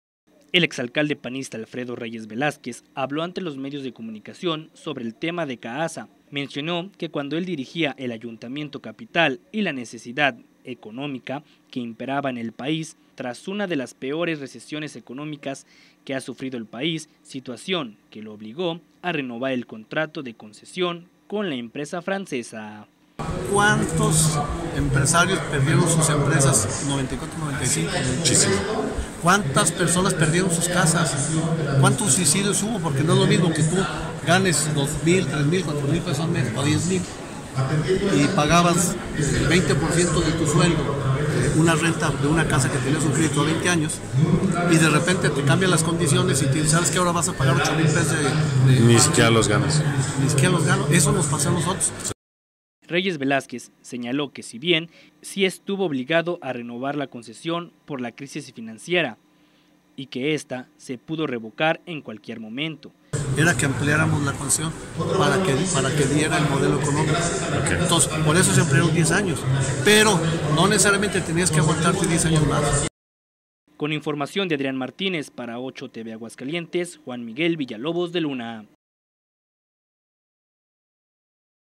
RENOVÉ LA CONCESIÓN A CAASA PORQUE LA SITUACIÓN DEL PAÍS ERA OTRA, DICE ALFREDO REYES
El ex alcalde señaló que la fuerte recesión que vivía el país, lo obligó a renovarla concesión con la empresa de capital francés.